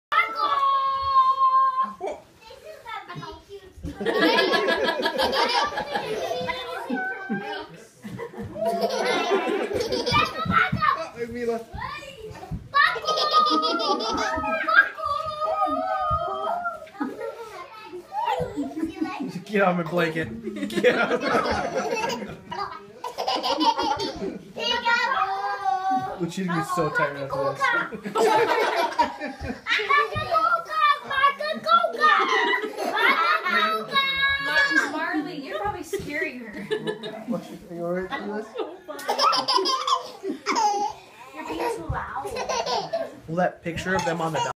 Making LuLu laugh
Cousins making her laugh at her second birthday party